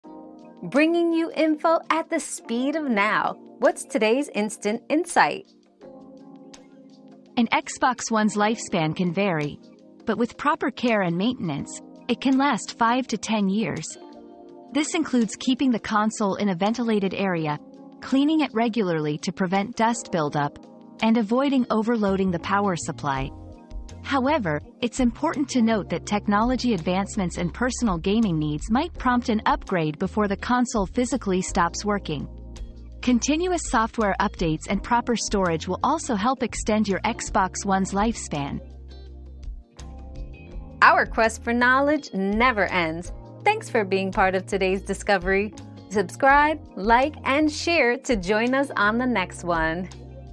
How long can a Xbox One last?
How Long Can Your Xbox One Last? 👉 Xbox One Lifespan 👉 Discover how to make your Xbox One last 5 to 10 years with proper care, maintenance, and software updates. Learn the tips to avoid overheating, dust buildup, and power supply overload for a longer gaming experience.

Laura S. Harris (2024, March 28.) How long can a Xbox One last?
    🌐 AskAbout.video